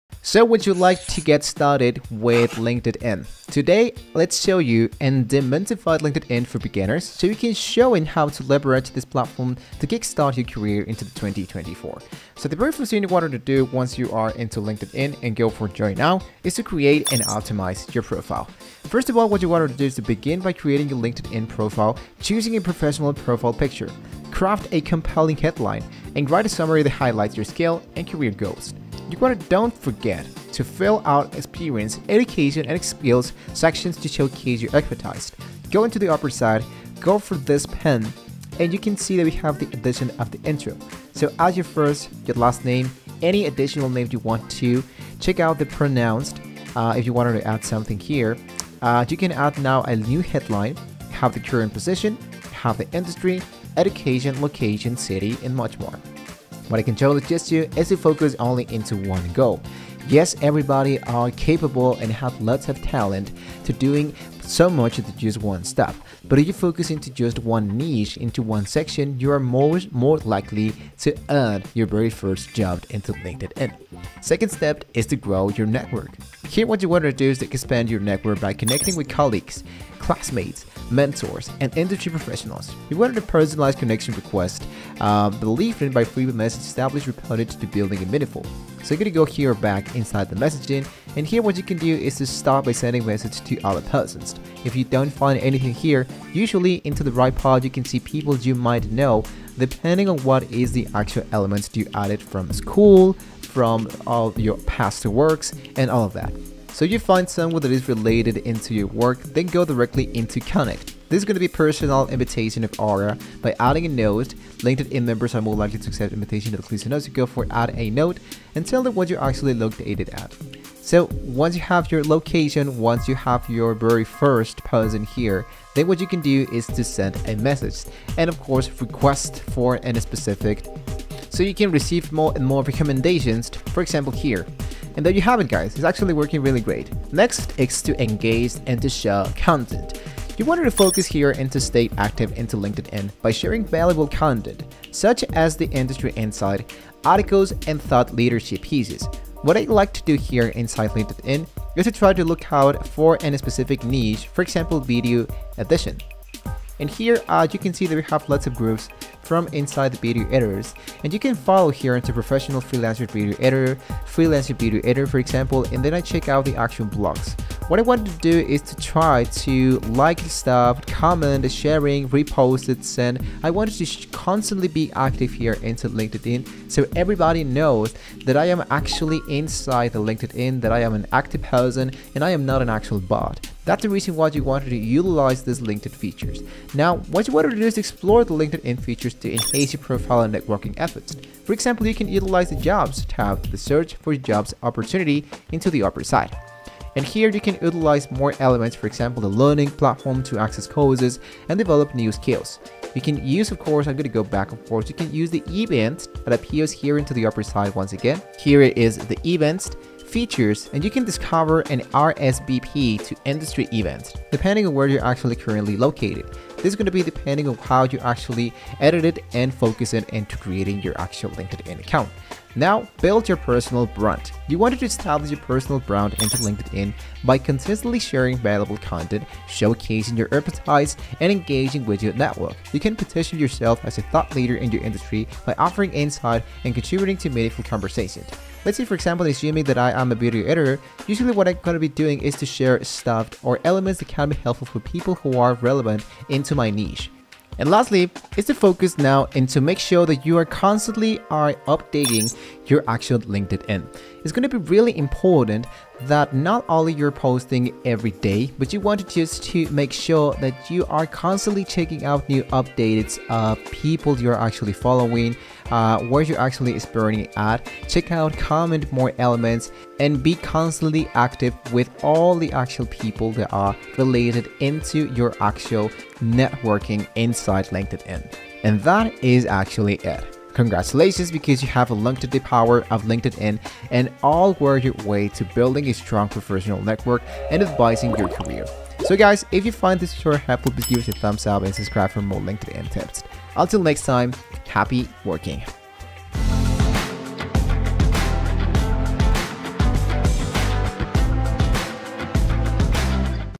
How To Use Linkedin For Beginners 2026 (Full Tutorial)
LinkedIn is a powerful tool for professionals to network, showcase their skills and accomplishments, and find new career opportunities. As a beginner, it may seem daunting to navigate this platform. However, by following a few simple steps, you can get started on LinkedIn with ease. Firstly, create a complete and professional profile that highlights your experience, education, and skills. Next, connect with people you know and join relevant groups to expand your network.

Engage with content, share your own insights, and participate in discussions to establish yourself as a thought leader in your industry. Lastly, utilize the job search feature and apply for positions that align with your career goals. With these tips, you can make the most out of LinkedIn in 2024 and beyond.